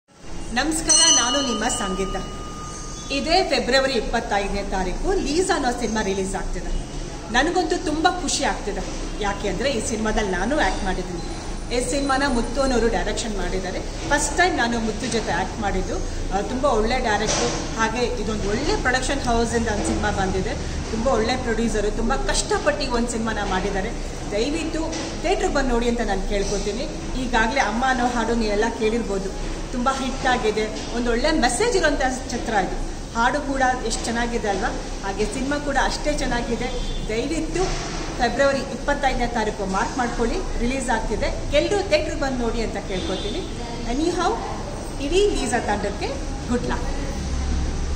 LISA - Video Bytes | Sangeetha Anil | PTK Production | Suryakanth Korali | Surendranath
Preasinting "LISA" Kannada Movie Grand Relasing On Feb 25th 2022, Directd By Muthu, Music By Surendranath, Lyrics By Thapasvi, Starring Roopa Nataraj, Parvathi, Saakshi, Shravya Ganapathi, Thejasvini,

Movie Name : Lisa
Banner : PTK Production
Director : Muthu,
Producer : Suryakanth Korali
Co Producers : Avinash Jhankatti, Devsree, Dayanand, Muthu
DOP : Cine tech Suri
Lyrics : Thapasvi
Starring : Roopa Nataraj, Parvathi, Saakshi, Shravya Ganapathi, Thejasvini,
Editor : Jnaanesh B Matad